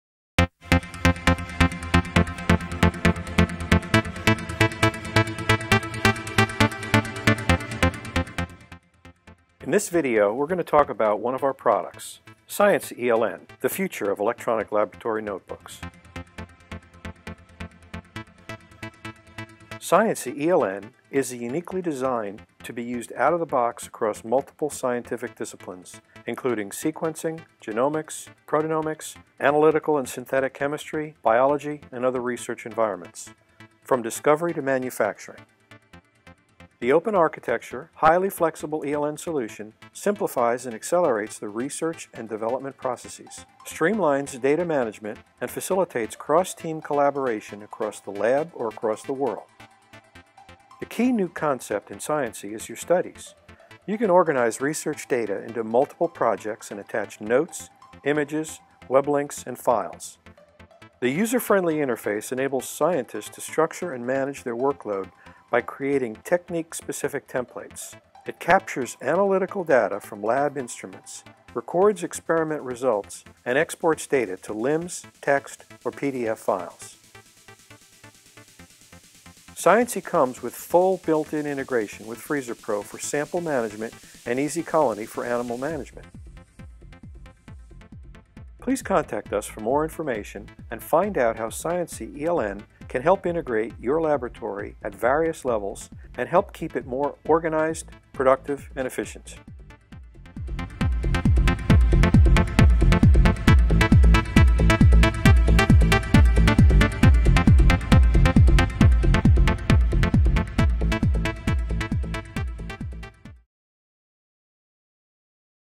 Sciency ELN - Electronic Laboratory Notebook
More than just "going paperless", Sciency ELN gives you a scalable, integrated research environment for managing and sharing data within a robust framework that protects valuable intellectual property, streamline laboratory processes and supports 21 CFR Part 11 compliance.